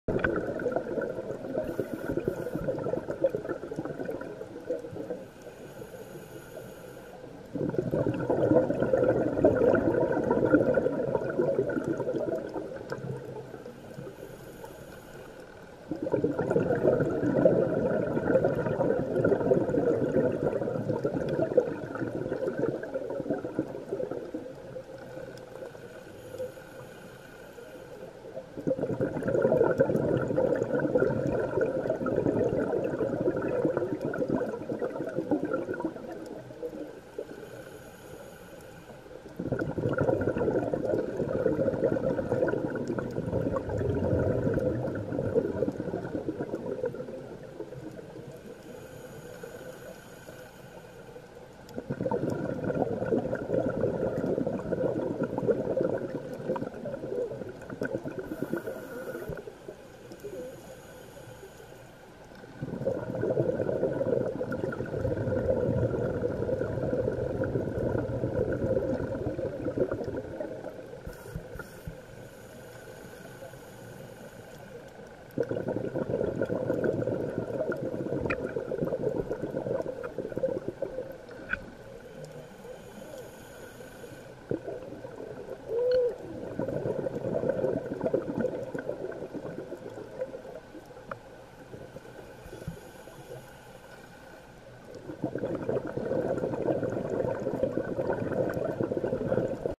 Sweet Lips Fish school
The Sweet Lips fish are a type of gunt. We see schools of grunts in the Caribbean, but the Asian Sweet Lips are more exotic.

This video was shot while diving with the Wakatobi dive resort in Indonesia, November 2019.